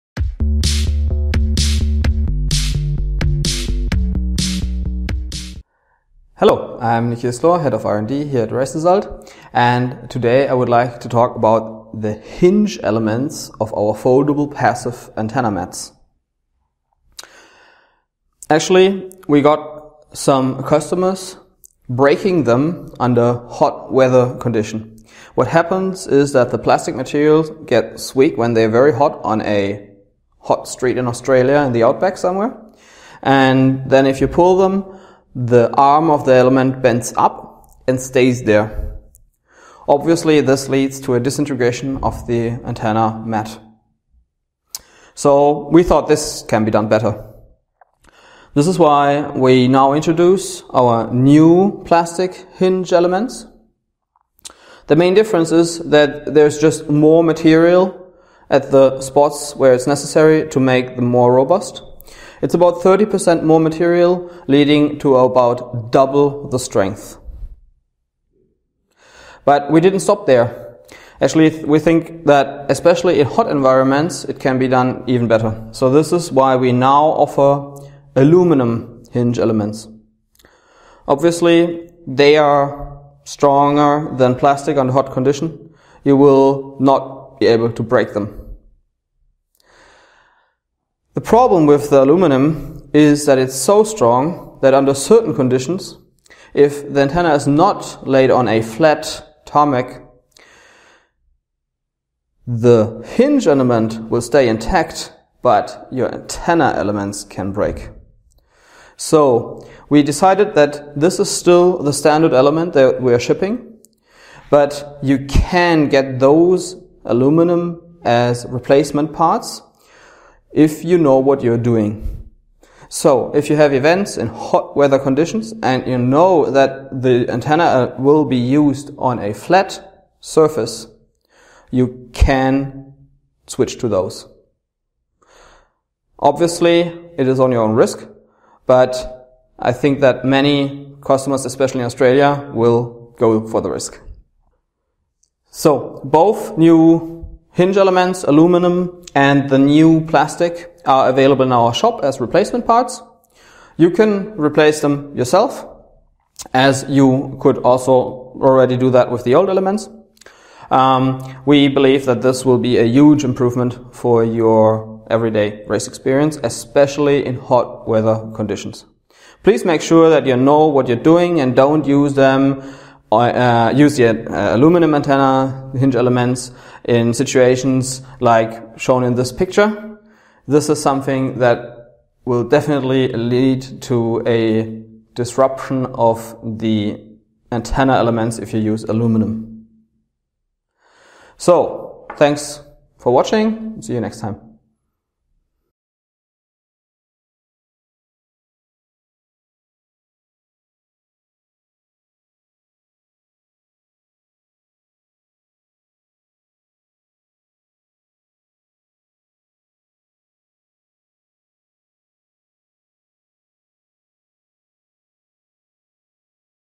race|result new Hinge Elements
We improved the hinge elements of our passive antenna. Watch this video and see the results!